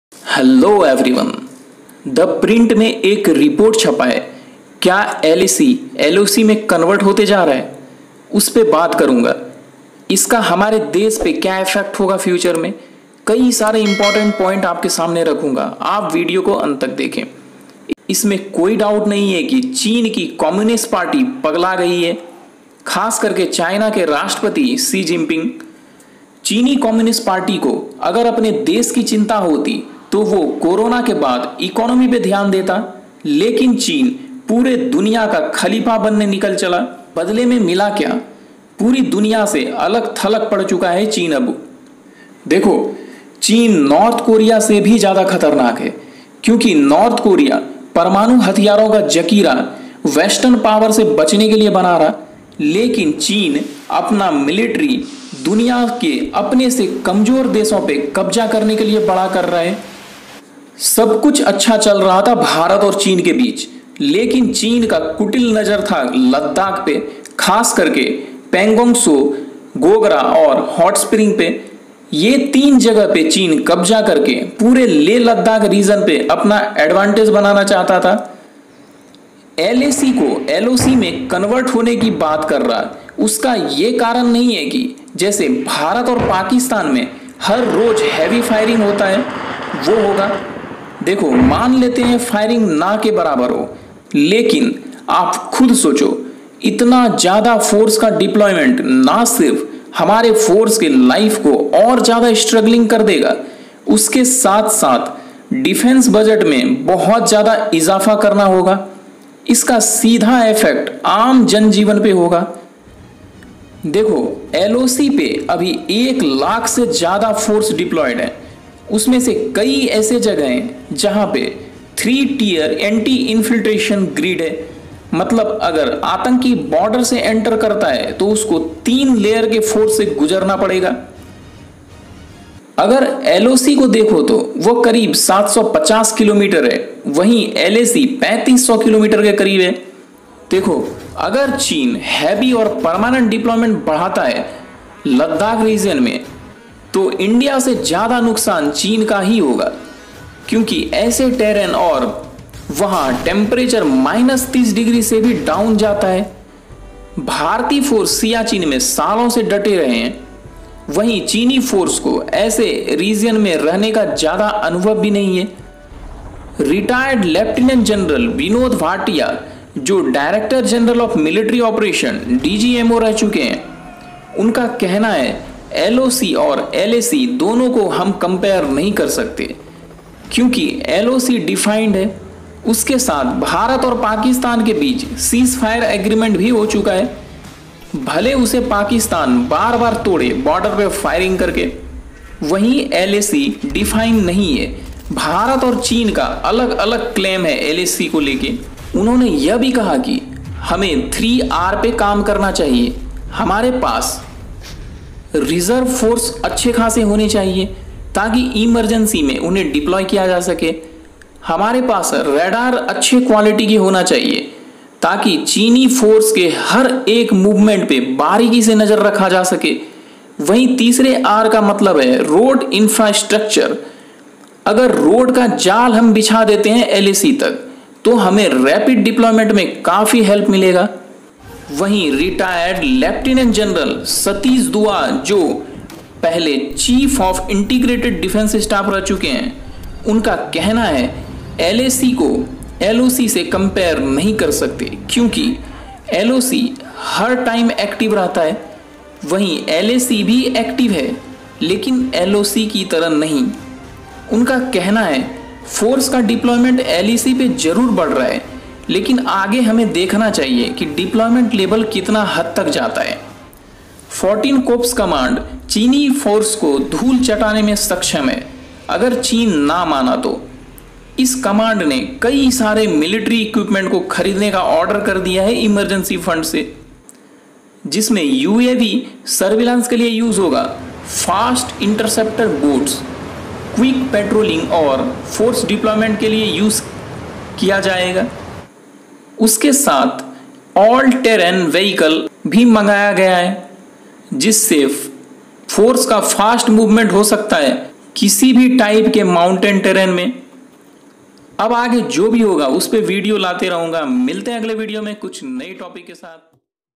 LAC turning into LOC?!- India China border fight- India China Conflict, India-China Standoff.
hello everyone
India china tension on verge . in this video you will get to know can line of actual control convert into line of control.

contact  my bro for logo and any graphic work , He is graphic designer.
Logo designed by Miraphic Design Co.